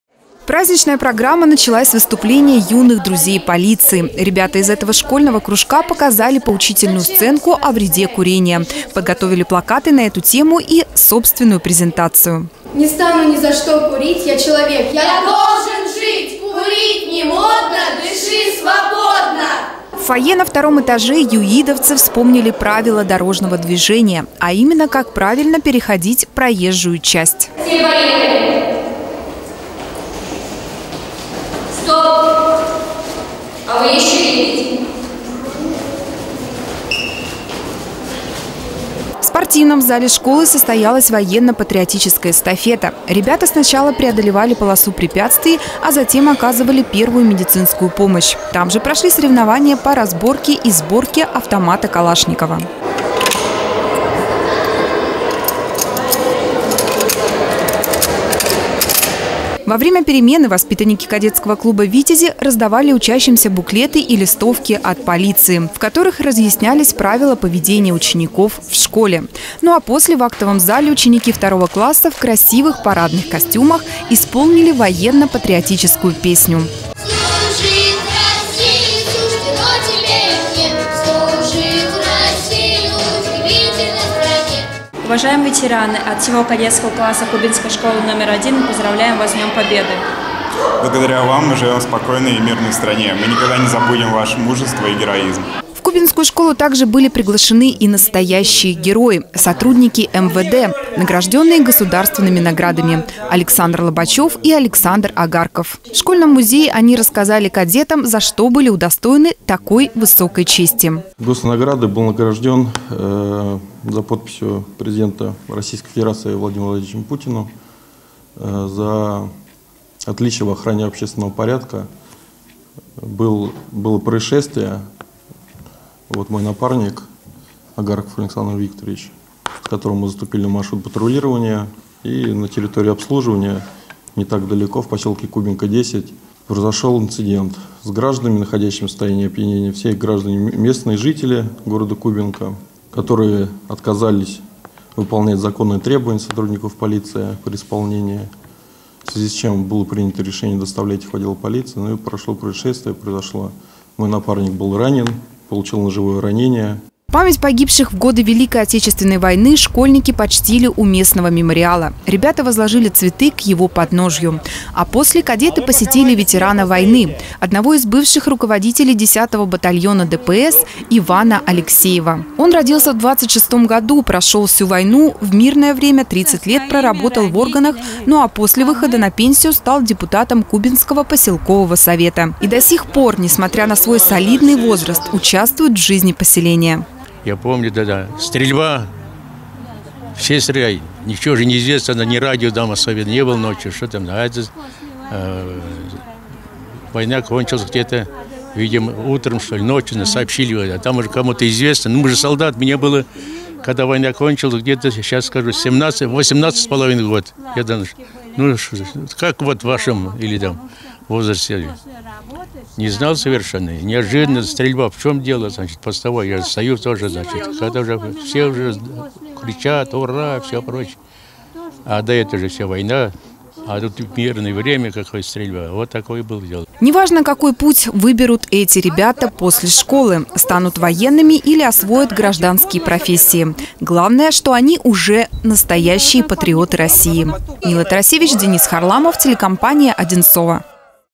Патриотическая акция в Кубинской школе
Ко Дню Победы воспитанники Кадетского клуба «Витязи» из Кубинской школы №1 подготовили целую праздничную программу. Военно-спортивная эстафета, выступление агитбригады, встречи с интересными людьми. Завершился этот насыщенный день – посещением жителя Кубинки, ветерана войны Ивана Алексеева.